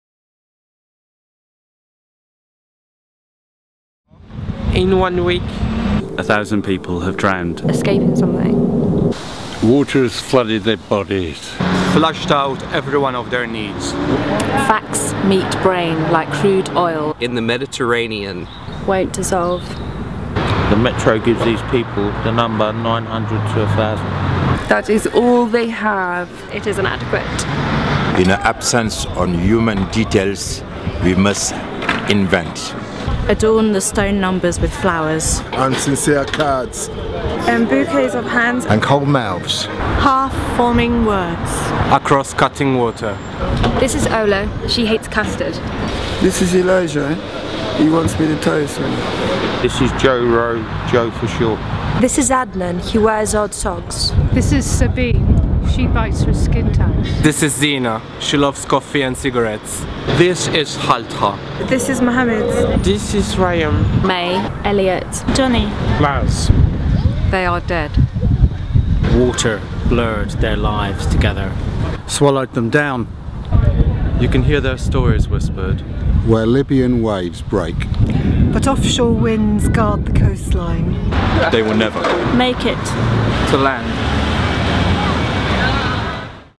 900-1000 Spoken Word
A poem performed by the members of the public addressing the media coverage of the tragic sinking of a boat carrying migrants across the Mediterranean.